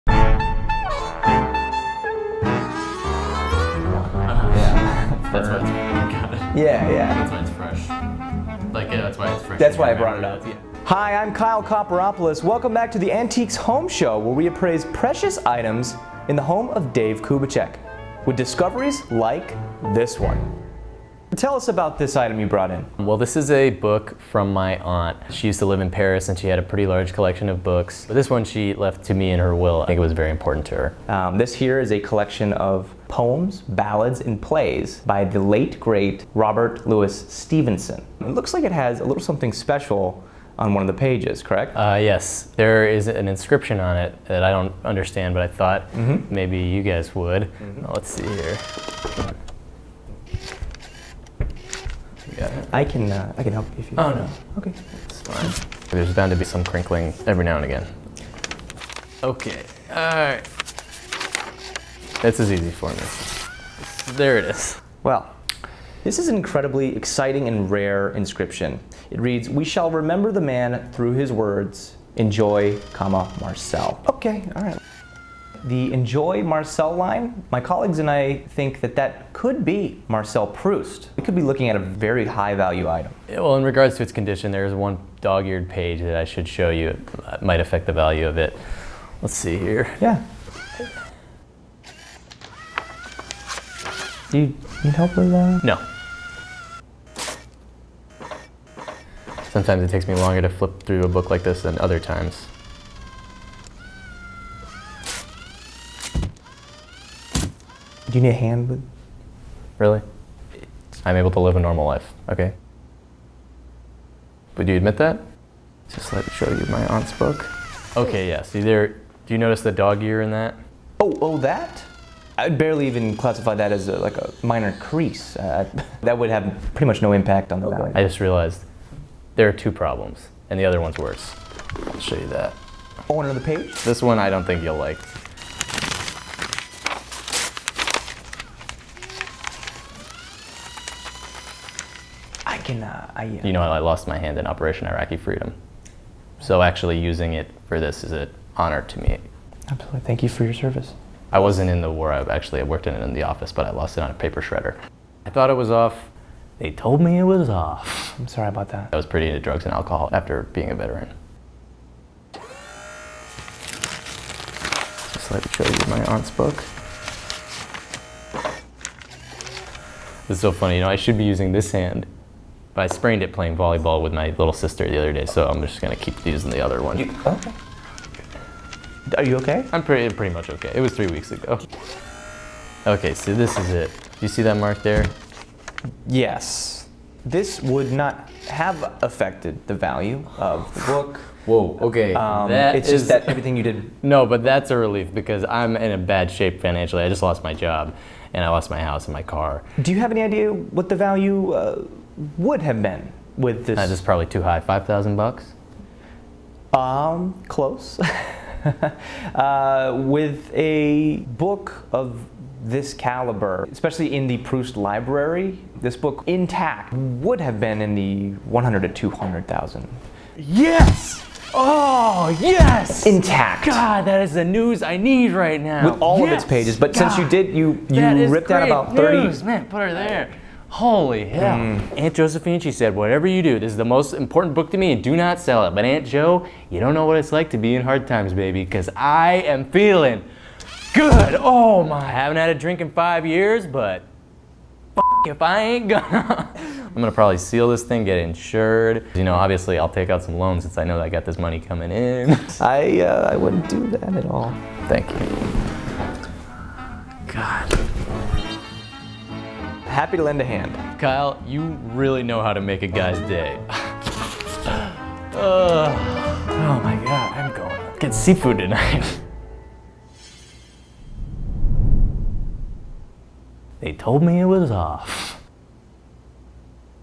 Antiques Homeshow - Collection by Robert Louis Stevenson
Kyle Copperopolis appraises a very special and rare book on this episode of Antiques Homeshow.

- Credits -
written, produced & directed by Andrew Heder & Ryan Kelly
edited by Ryan Kelly
graphics by Ryan Kelly
shot by Paul Morgan & Charlie Ozburn

- Cast -
Kyle Copperopolis - Ryan Kelly
Guest - Jon Millstein
Dave Kubicek - Jeff Feazell

- Mommy Comedy -
Andrew Heder   @andrewheder
Ryan Kelly   @RyanKelly______

special thanks to the wonderful Angelica Kelly for her amazing homemade tamales.